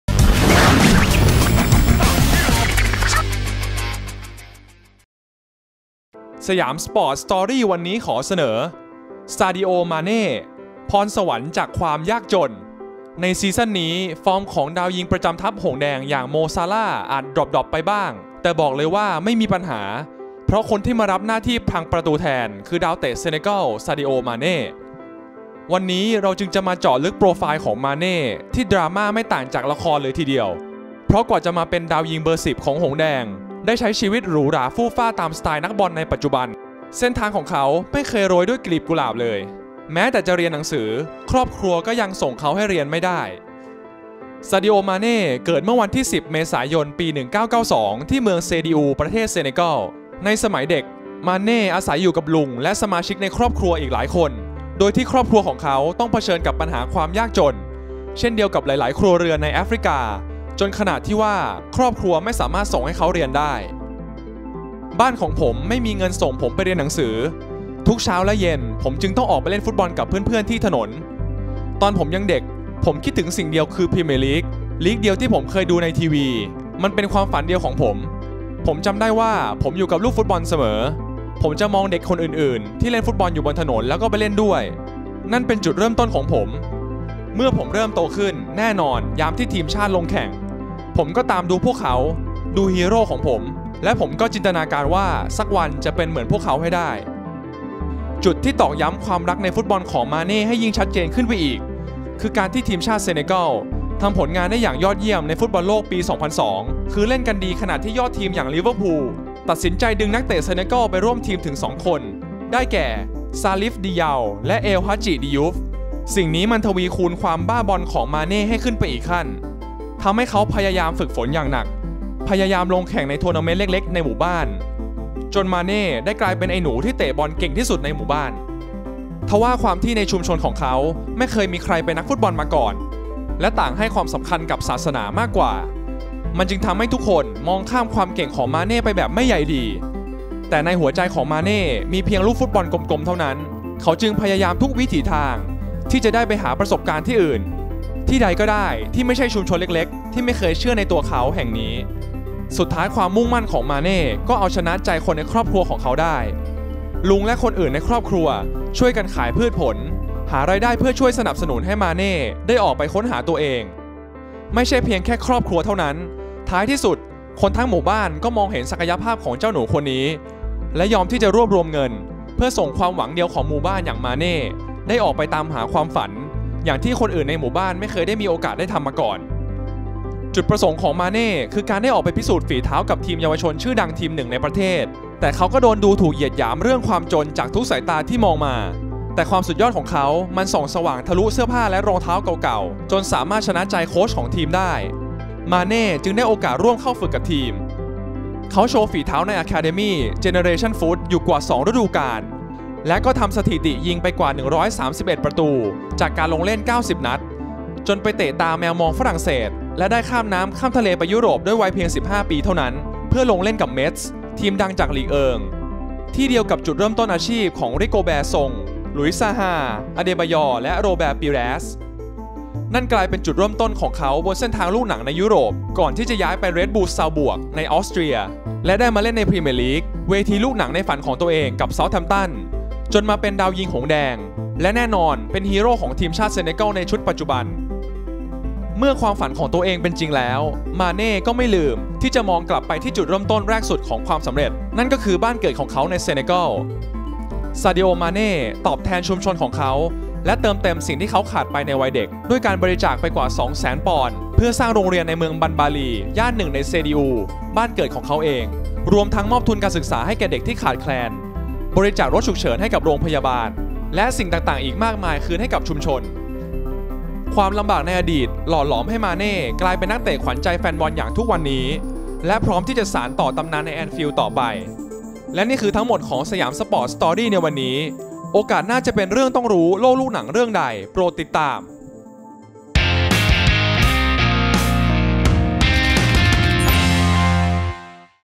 ซาดิโอ มาเน่ พรสวรรค์จากความยากจน | Siamsport Story
รู้หรือไม่? กว่าจะก้าวมาเป็นยอดแข้งในปัจจุบัน ซาดิโอ มาเน่ ต้องเจอกับอะไรมาก่อน? ไปหาคำตอบได้กับประวัติจัดเต็มของ มาเน่ ในรายการ Siamsport Story